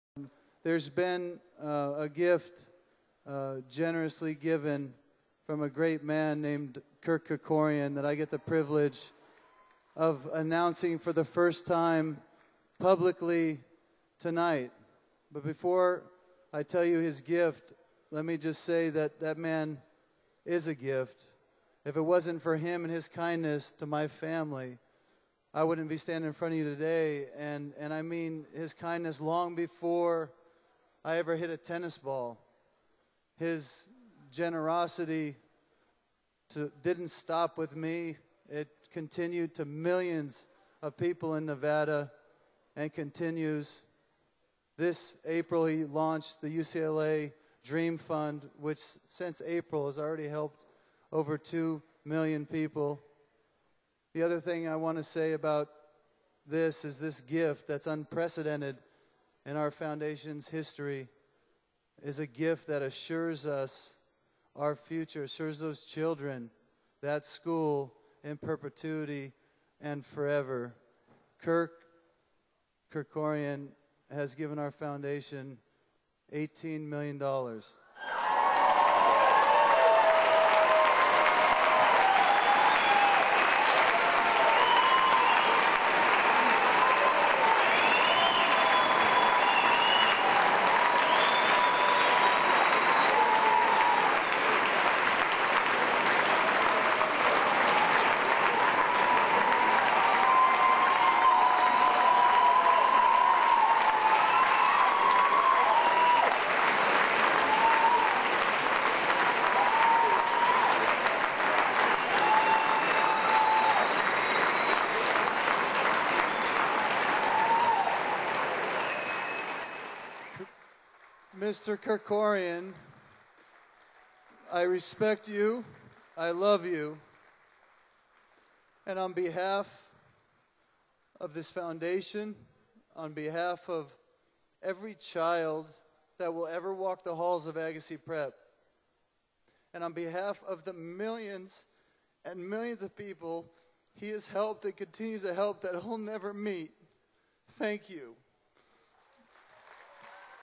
Andre Agassi Announces Kirk Kerkorian Donation
Video Footage pulled from News Market and provided by the staff of getupradio.com.

Tennis legend and philanthropist Andre Agassi hosted the 2011 Grand Slam for Children on Oct. 29 at Wynn Las Vegas. Michael Bublé, Jimmy Kimmel, Martina McBride, Smokey Robinson and Train headlined the 16th annual benefit concert. The event benefits the Andre Agassi College Preparatory Academy, a tuition free K-12 public charter school in Las Vegas.